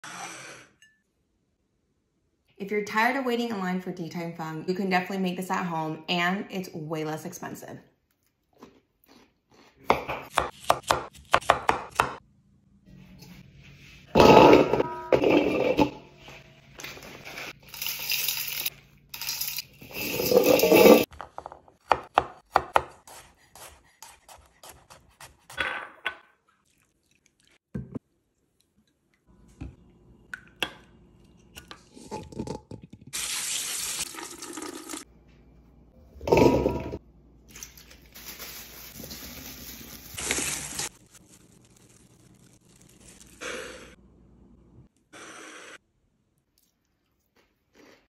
Copycat Din Tai Fung CUCUMBER SALAD - make yours at home - Gluten-Free & Vegan
EP. 16 of the Asian Recipe Series - Copycat Din Tai Fung CUCUMBER SALAD 🥒 before all those Persian cucumbers go bad from the big bag you purchased, MAKE THESE. It’s so simple, refreshing and delicious to snack on. Gluten-free, Sugar-Free and Vegan.

6 persian cucumbers - cut ½” thick 1 tbsp sea salt (I like to use course salt)
2 tbsp rice vinegar1 tbsp allulose sweetener or agave
1 tbsp water 1 tbsp chili oil 4 garlic cloves - thinly sliced
2 tbsp gf soy sauce
1 tsp sesame oil

Mix the cucumbers and salt together in a large bowl and let it sit for 30 - 45 minutes. Rinse with water 3-4 times, then pat dry really well.
Mix sauce ingredients for the cucumber salad dressing. Toss the rinsed cucumbers with the dressing and serve immediately.